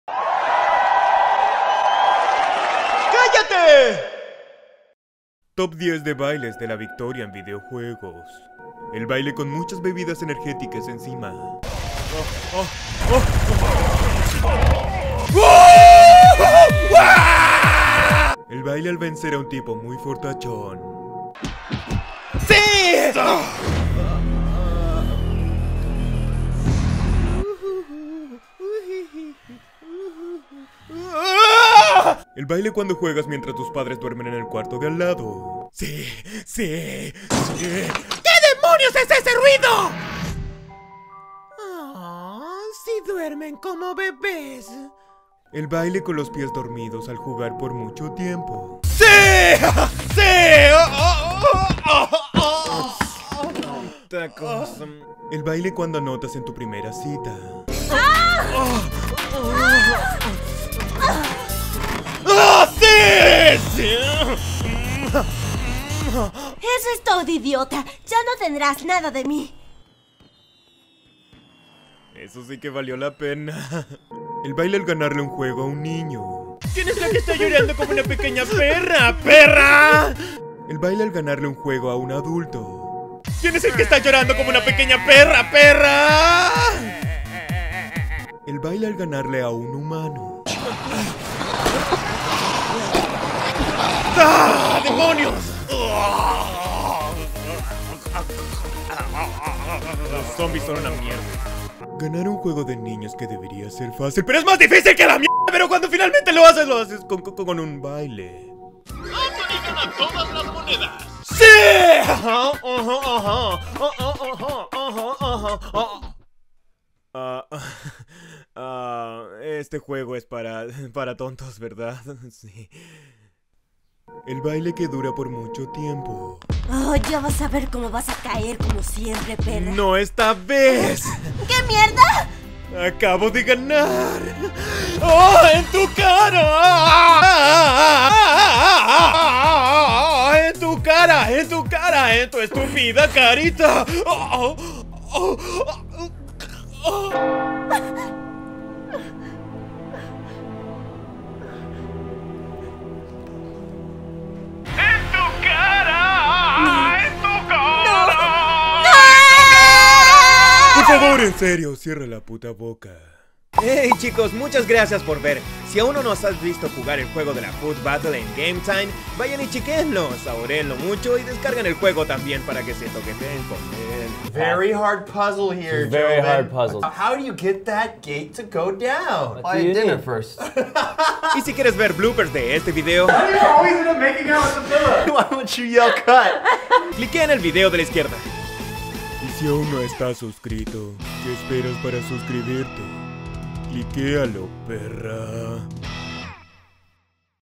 TOP 10 DE BAILES DE LA VICTORIA EN VIDEOJUEGOS
Cada victoria merece su propia y única celebración. Aquí están 10 de las mejores celebraciones que conocemos al ganar un videojuego. La próxima oportunidad te cargaras a tu amigo n00b con uno de estos suculentos bailes.

Cast
Ian Hecox
Anthony Padilla
Tanner Murrietta as Buff Guy
Brittni Barger as Girlfriend

Cast de Doblaje
Ian/Anthony: Patrick Maulén (Chile)
Novia: Kai (Venezuela)

Written by: Anthony Padilla & Ian Hecox
Produced & Directed by: Anthony Padilla, Ian Hecox, & Ryan Todd
Edited by: Michael Barryte
Post Supervision by: Ryan Finnerty

Coordinating Producer: Taryn McDonough | DP: John Jimenez
Camera Op: Jon Joiner | AC: Brendan Day | Sound: Ivan Harder
Production Designer: Odin Abbott
Grips: Patrick Egan & Lee Eisenhower
Wardrobe: Felicia Cowley | Makeup: Paula Barkley
PA: Ryan Sweeney | Art Dep. PA: Steve Geogerian
DIT/Media Mgmt: Gabe Laguer
Color: Pretty Moving Pictures | Asst Editor: Katie Reed
BTS: Phil Mohr | Casting: Brian Jagger
aMFandub

¡Mira! Es nuestro propio sitio web
¿Quieres saber cuando estamos filmando y/o cagando? Ahora puedes